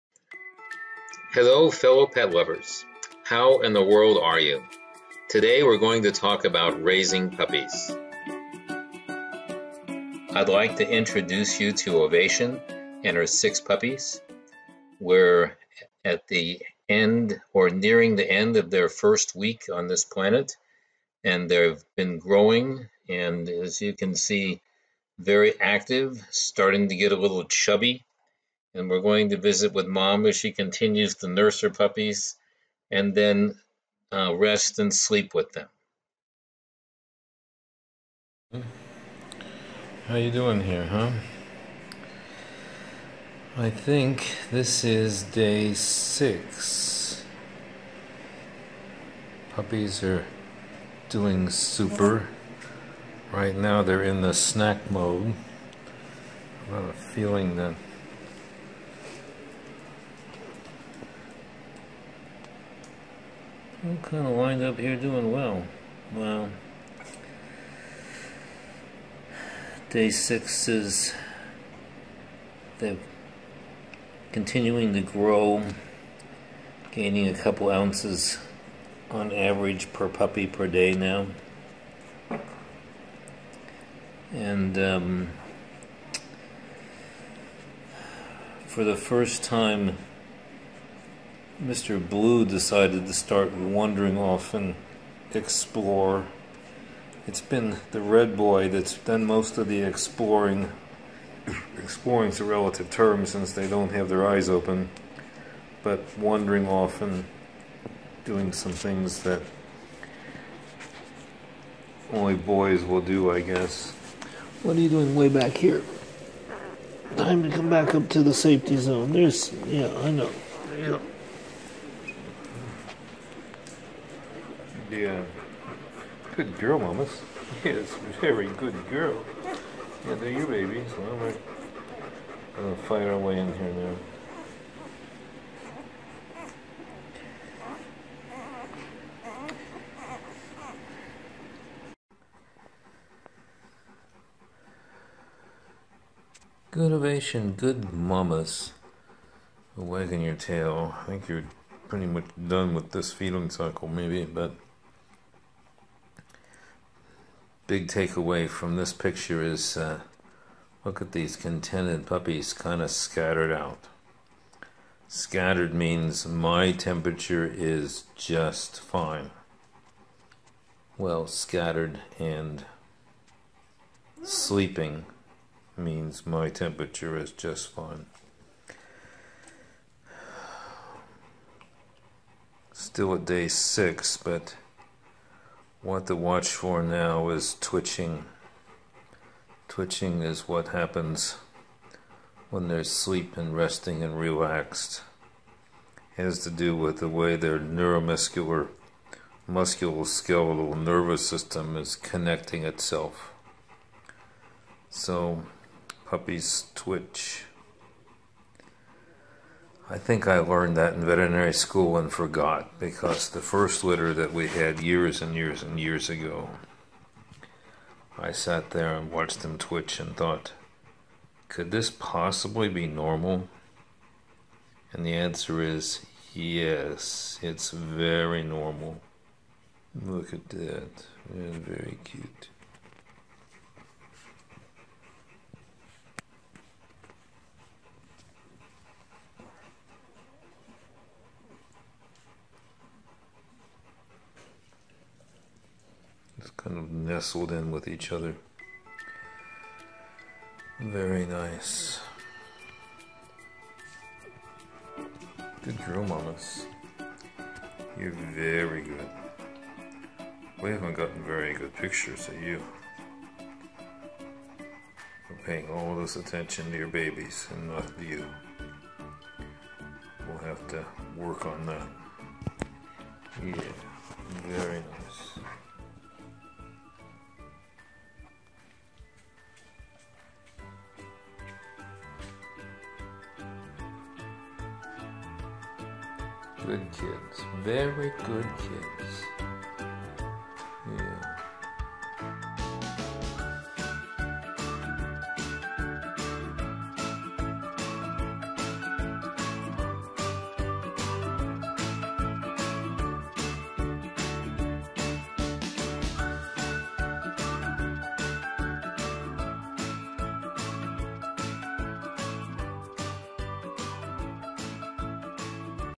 Why Do Newborn Puppies Twitch? - Ovation's Puppies
In this video we see puppies twitching. At first blush if we're not prepared, this looks scary and violent as if they're starting to have a seizure.    Interested ??

 But this is very normal. It is a full-body neuromuscular activity,  which we now understand to be associated with their central nervous system developing and connecting with itself and their musculature.

Now I find it relaxing and calming ! ... Dr.B.

What about you!    Interested ??